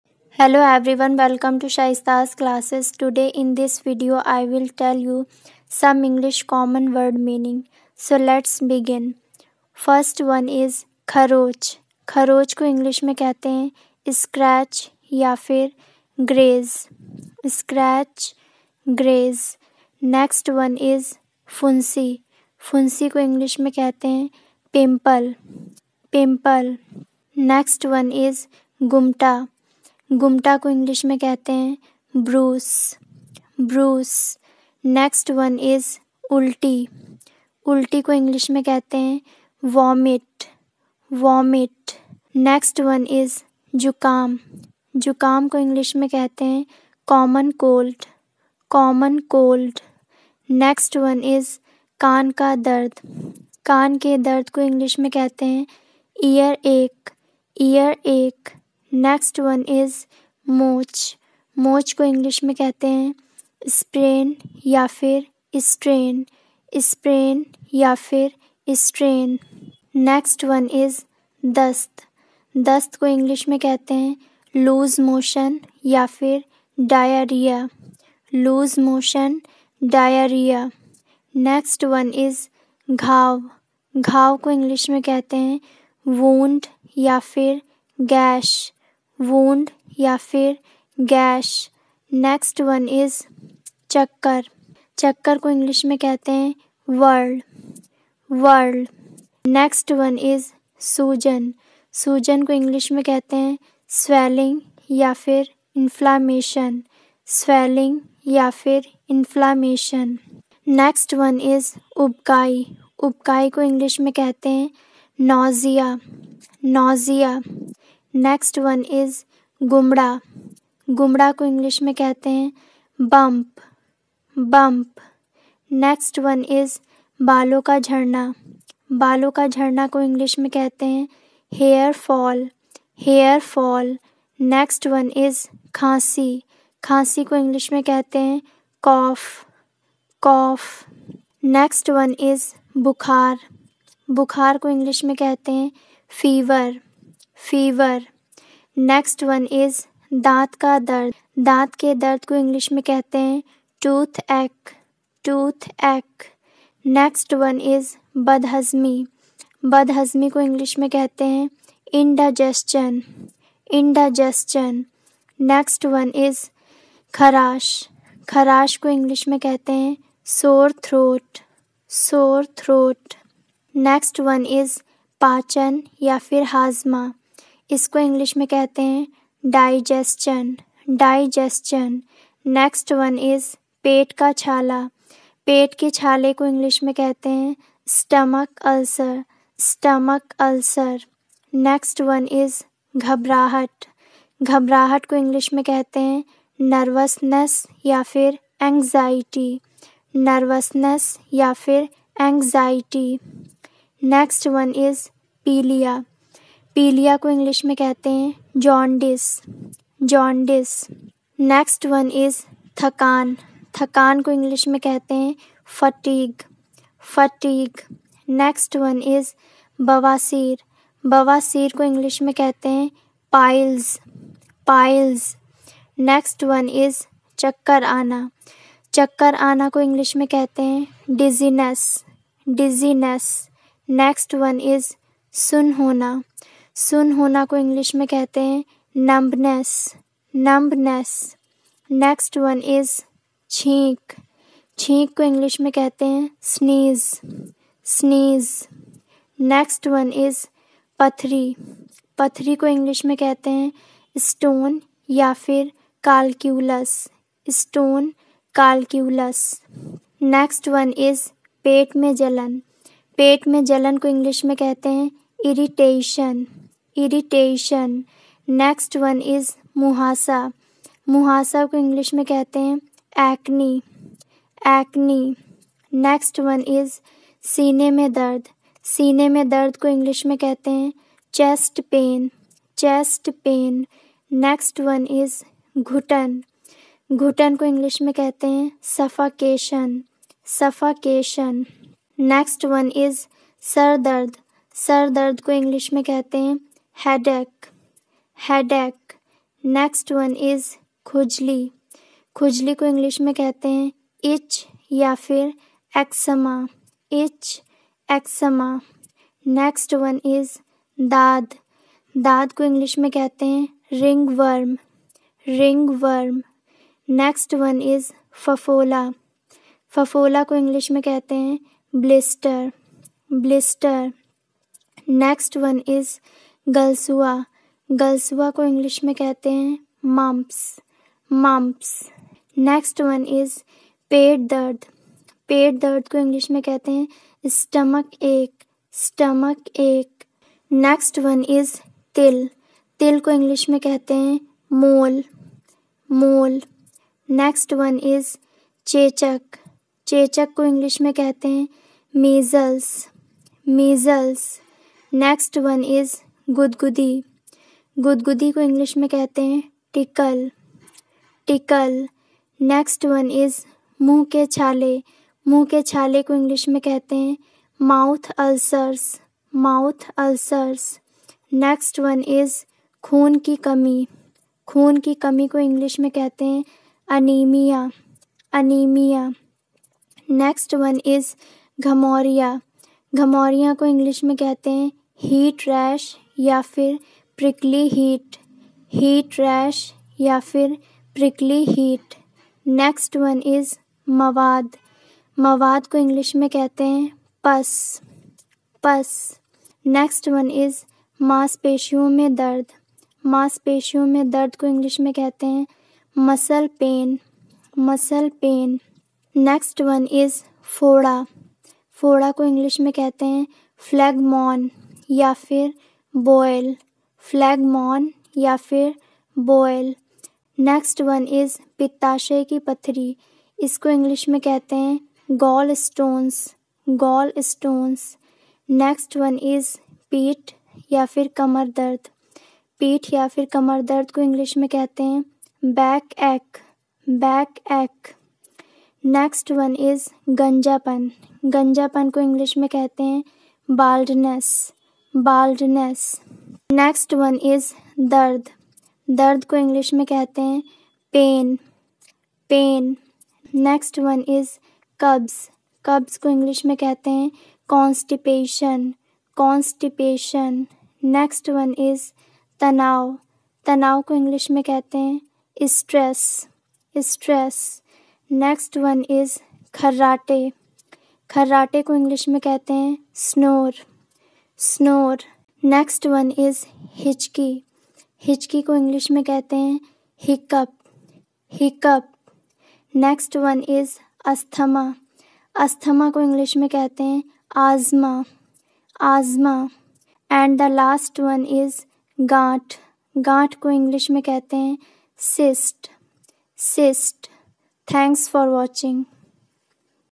Common English Words With Hindi Meaning।। Daily English Speaking Word Meaning।। English Vocabulary.
About this video 👇

In this video you will learn 59 english common word meaning with pictures and correct pronunciation,if you learn all these, you use all the words in your daily life then you can enhance your english knowledge so watch this video till the end if my video is useful for you then like, share my video and subscribe to my channel,Thank you.